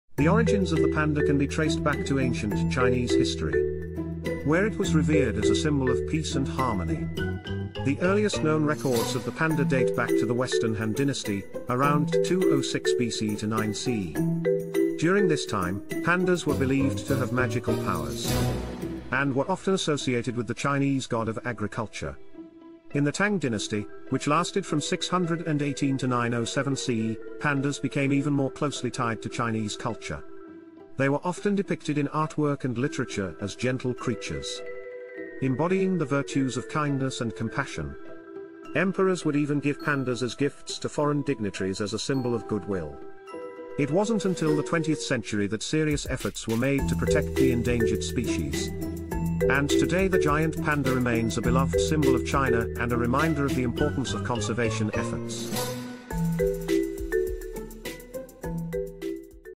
The Mystical Origins of Pandas: Ancient Esoteric Secrets
In this video, Lost Libraries explores the enigmatic history of the panda, a beloved creature in modern times but with a mystical past. From ancient Chinese legends to esoteric symbolism, we delve into the secrets of the panda and its place in the spiritual and cultural traditions of the past. Join us as we unravel the mysteries of the panda's mystical origins.